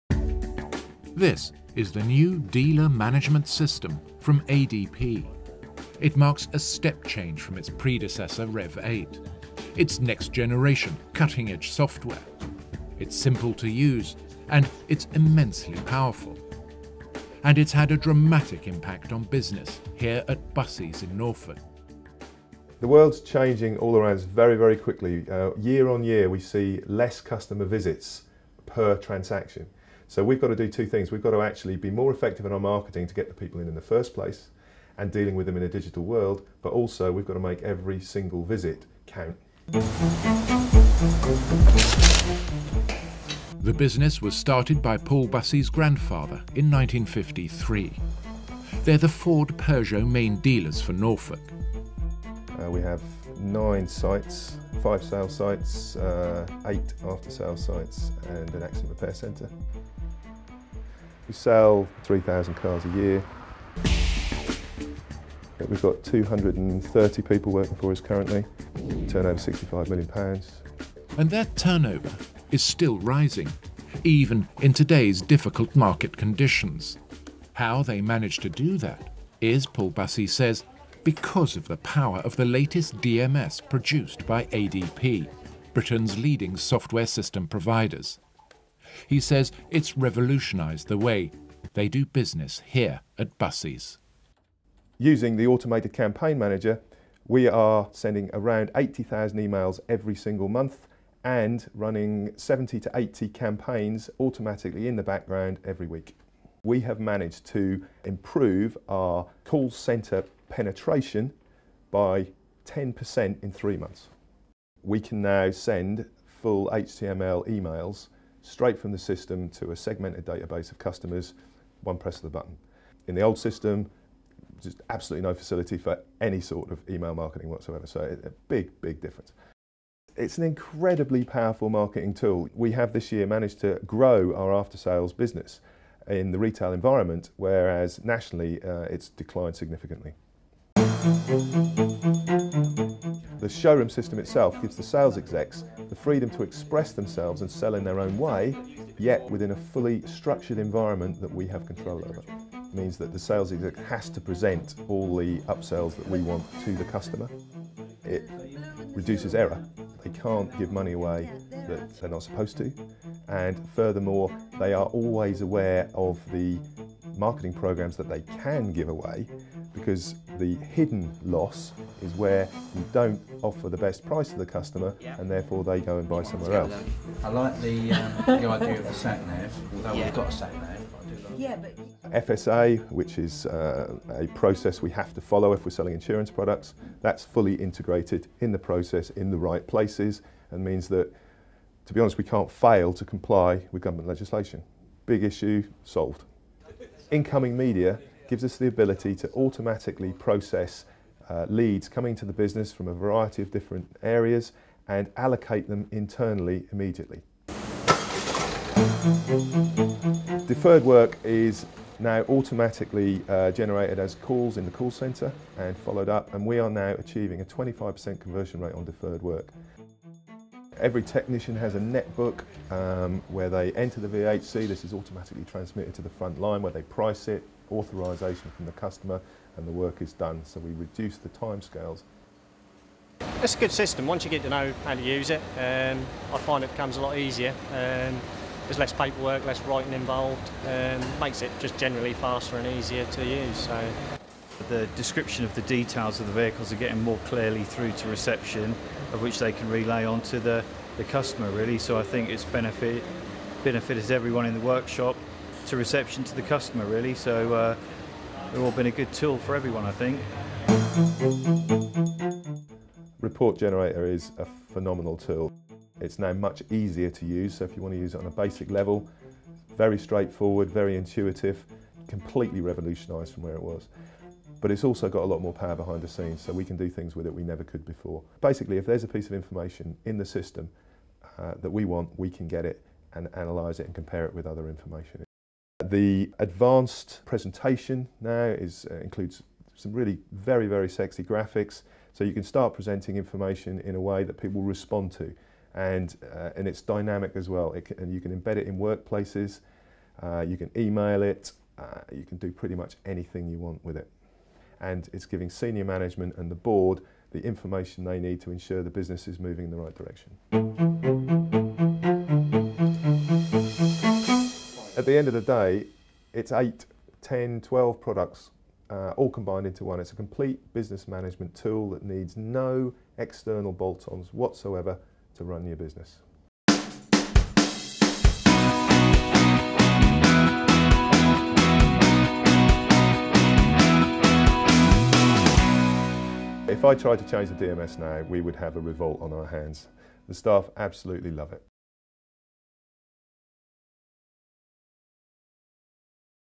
ADP
Busseys are the Ford Peugeot Main Dealer for Norfolk. At a time when most companies are finding it hard to maintain their bottom line, Busseys turnover is increasing. Paul Bussey says this is down to cutting edge software which provides extremely effective automated dealer management systems. Produced for ADP by Adam Palmer & Rostand Productions.